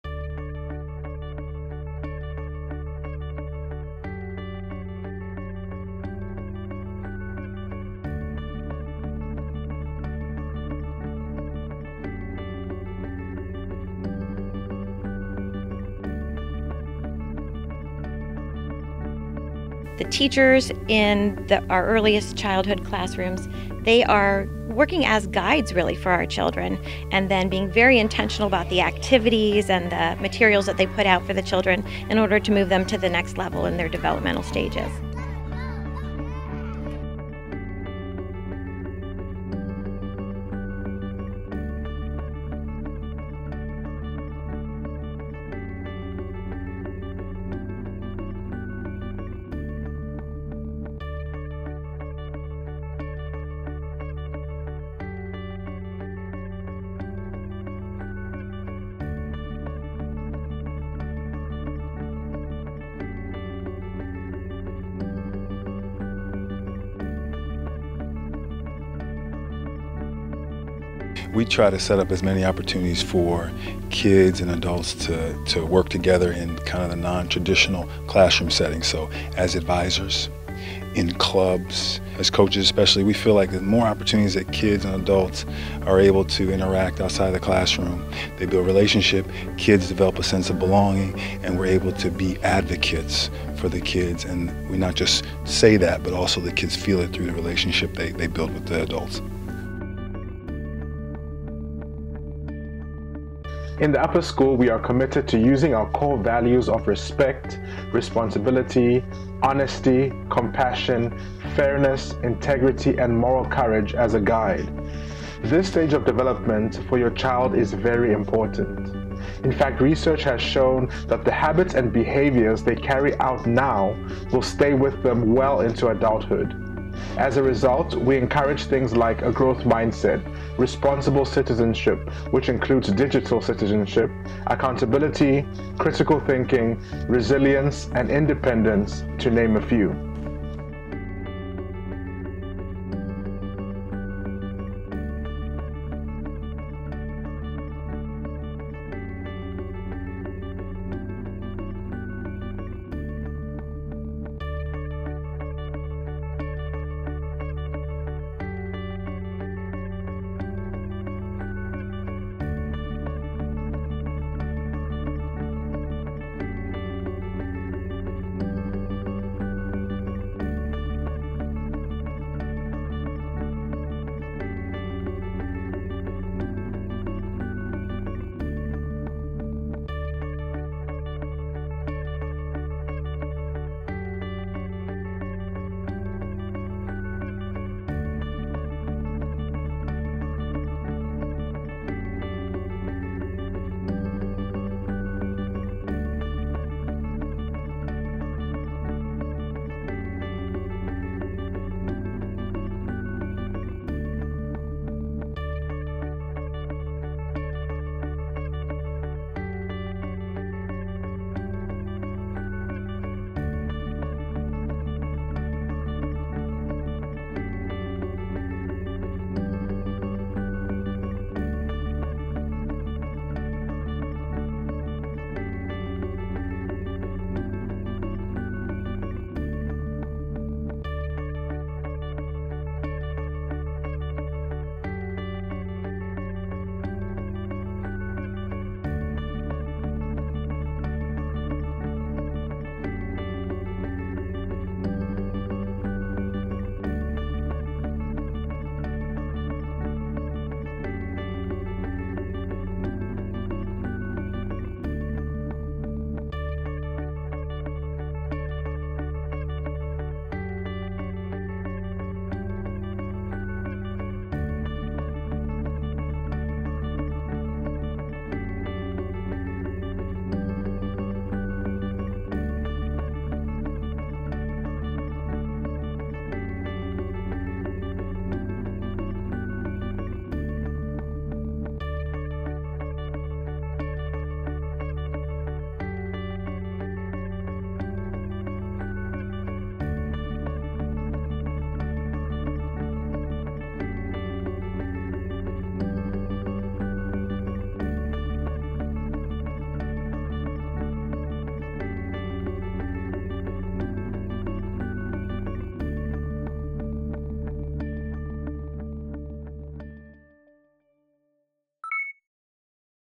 Columbus Academy Virtual Tour
This five-minute virtual tour of Columbus Academy's 231-acre campus will give you a glimpse into the many features and happenings that make up the Academy Experience! *All photos and video footage of students, teachers and various school-related activities were taken/recorded prior to the COVID-19 pandemic and Columbus Academy's reopening under the 2020-21 school year's health and safety guidelines.